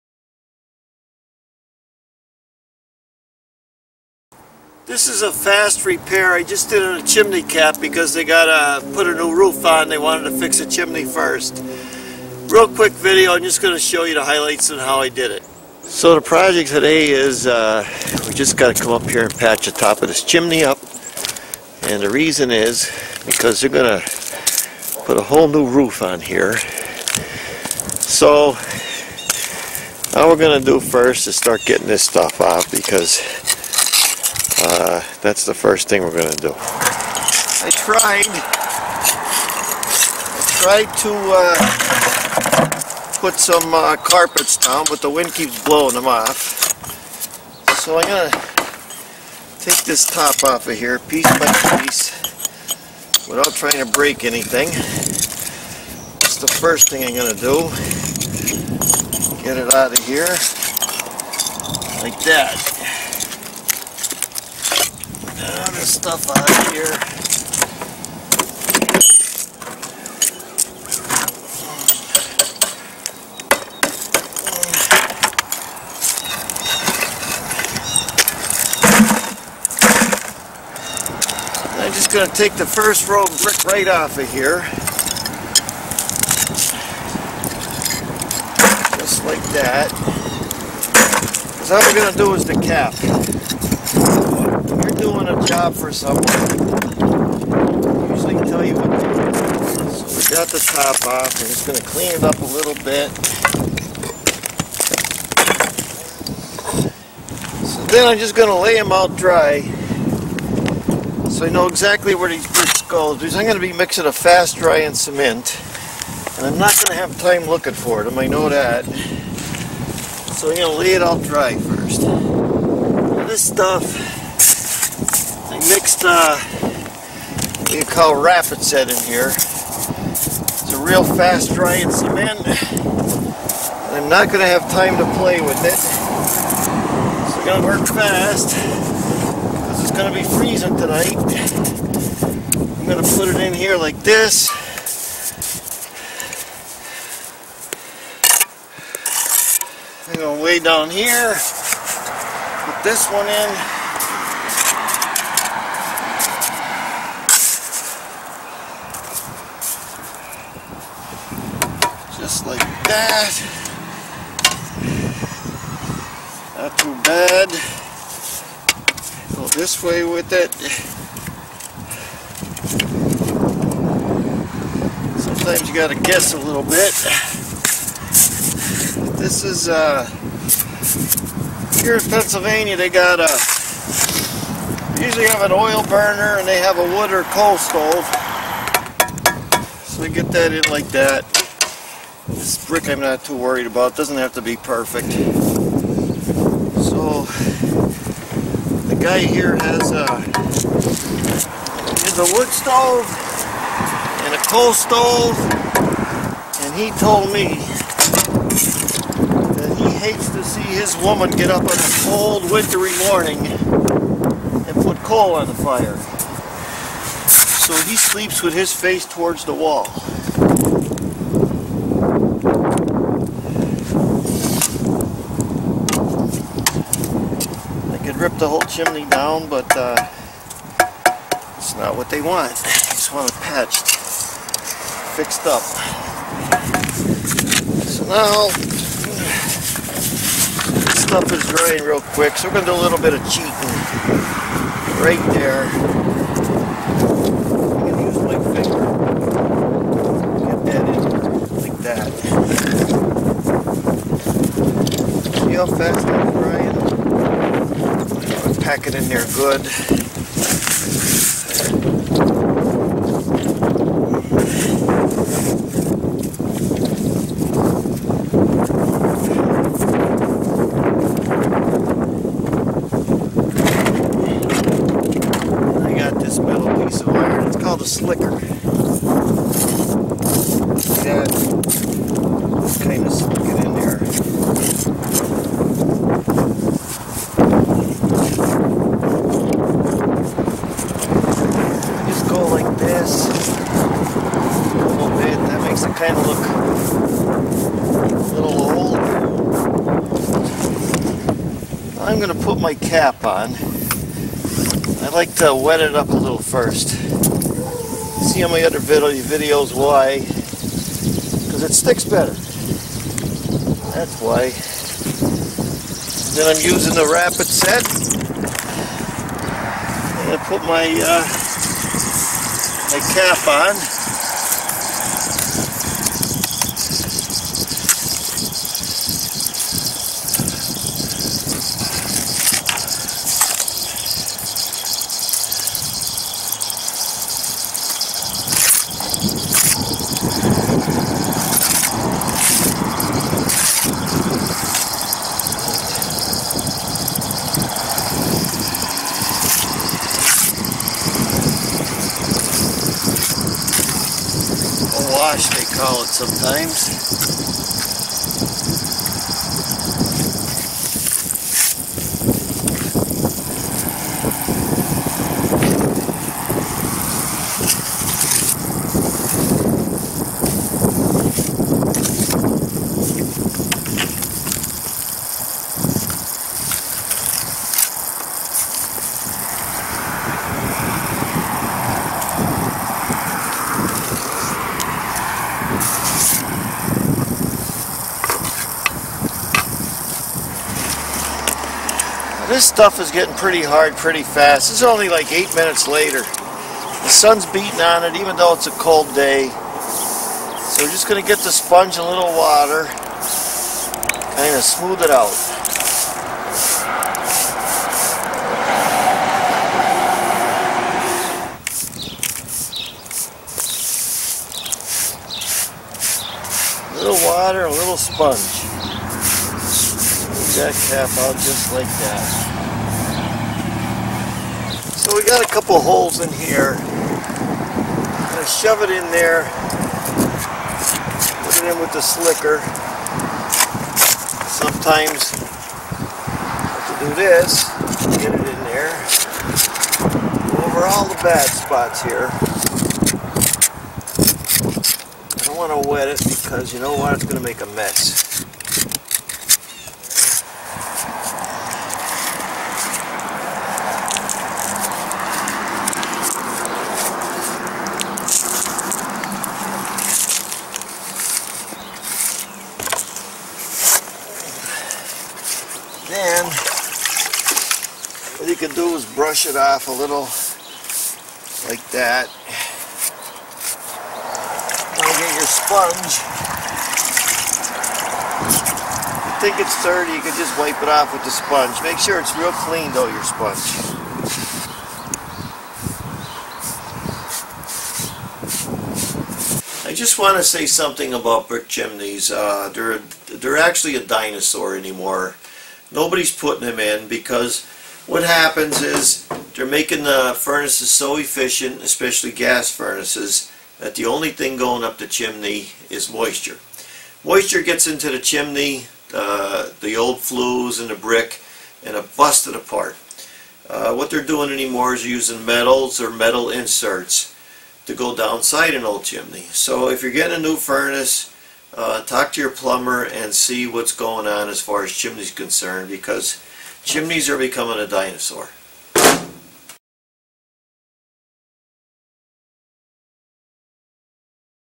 CHIMNEY REPAIR (part 3) Mike Haduck
Mike Haduck shows how he does a fast repair on a brick chimney cap before the new roof is installed. all my videos are my ways and ideas, I always suggest anyone doing any type of work to consult profession help. tools etc